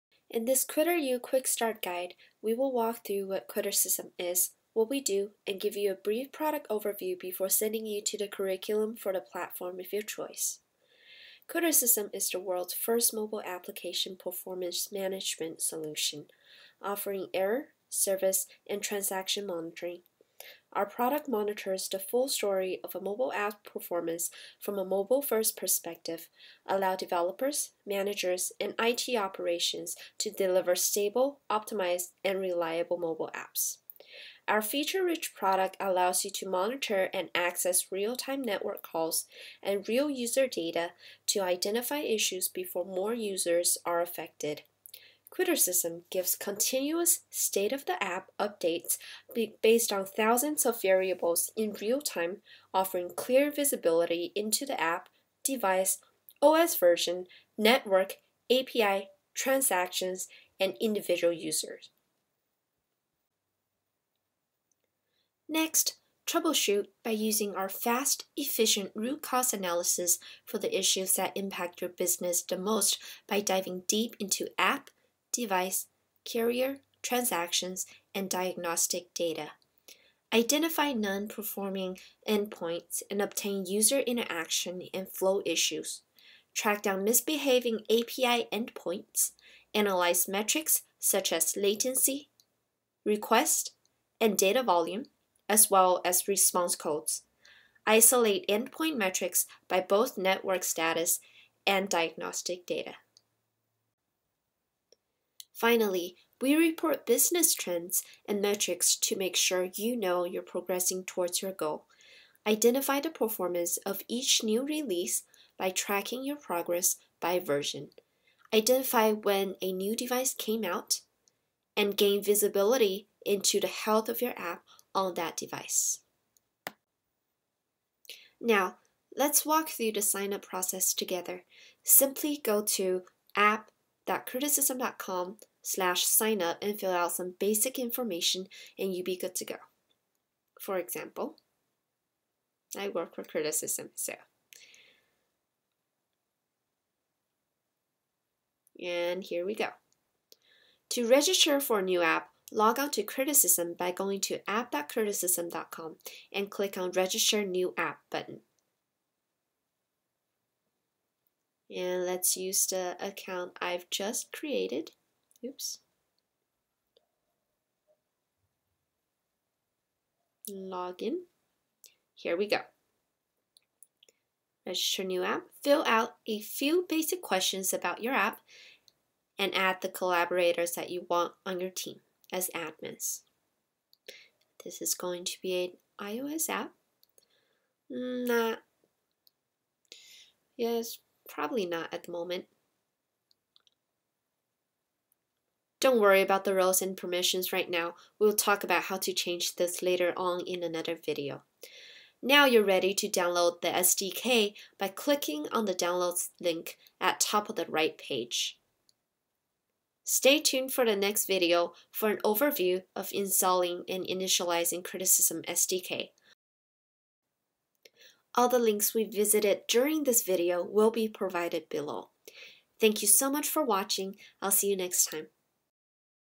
Critter U QuickStart Guide
Crittercism is the world’s first mobile application performance management (mAPM) solution; offering error, service, and transaction monitoring. Our product monitors the full story of a mobile app’s performance from a mobile-first perspective, allowing Developers, Managers, and IT Operations to deliver stable, optimized and reliable mobile apps.

Our feature rich product allows you to monitor and access real-time network calls and real-user data to identify issues before more users are affected. Crittercism gives continuous “state of the app” updates based on thousands of variables in real time offering clear visibility into the app, device, OS version, network, API, transactions, and individual user.

You can also identify and prioritize business critical issues by size of impact and prioritize by users affected. Track the stats on different versions of your app across all mobile devices, and get insight into your production successes and those parts where code still needs work.

Next, troubleshoot by using our fast, efficient root cause analysis for the issues that impact your business the most by diving deep into app, device, carrier, transactions, and diagnostic data. Identify non-performing endpoints and obtain user interaction and flow issues. Track down misbehaving API endpoints, analyze metrics such as latency, request and data volume, as well as response codes. Isolate endpoint metrics by both network status and diagnostic data.

Finally, we report business trends and metrics to make sure you know you’re progressing towards your goal. Identify the performance of each new release by tracking your progress by version. Identify when a new device came out, and gain visibility into the health of your app on that device.

Now, let’s walk through the signup process together. and fill out some basic information and you’ll be good to go!

To register for a new app, log onto Crittercism by going to app.crittercism.com, and click on REGISTER A NEW APP button. Fill out a few basic questions about your app and add the collaborators that you want to your team as admins. Don’t worry about the roles and permissions right now, we’ll talk about how to change this later on in another video.

Now you’re ready to download the SDK by clicking on the Downloads link at top right of the page!